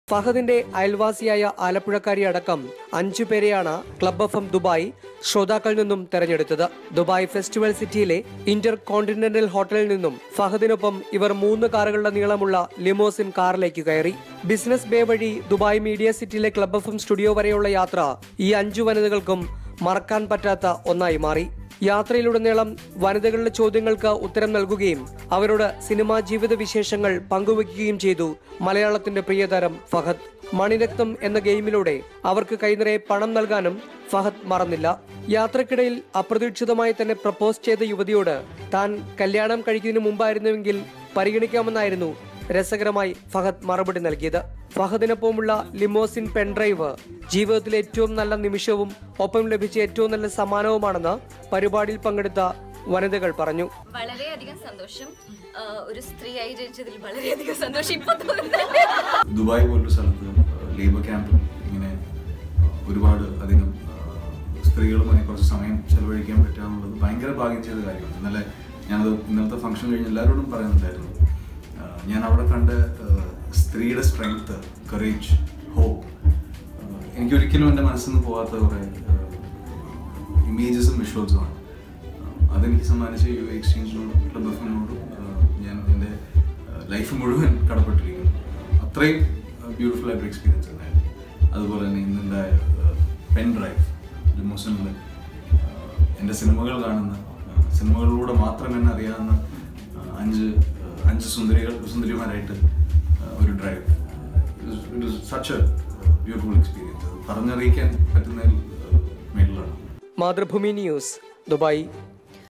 ദുബായില്‍ ലിമോസിനില്‍ ഫഹദ് ഫാസിലിനൊപ്പം ചുറ്റിക്കറങ്ങി സുന്ദരികള്‍
ദുബായ്: നടന്‍ ഫഹദ് ഫാസില്‍ ദുബായില്‍ അഞ്ച് സുന്ദരികളുമായി ലിമോസിന്‍ കാറില്‍ ചുറ്റിക്കറങ്ങി. ക്ലബ് എഫ്.എം ശ്രോതാക്കളില്‍ നിന്നും തിരഞ്ഞെടുക്കപ്പെട്ട അഞ്ച് വനിതകളാണ് പ്രിയതാരത്തിനൊപ്പം ആഡംബരയാത്ര നടത്തിയത്. വനിതാദിനവുമായി ബന്ധപ്പെട്ട് യു.എ.ഇ എക്‌സ്‌ചേഞ്ചാണ് പെന്‍ഡ്രൈവ് എന്നപേരില്‍ പരിപാടിയ്ക്ക് ചുക്കാന്‍ പിടിച്ചത്.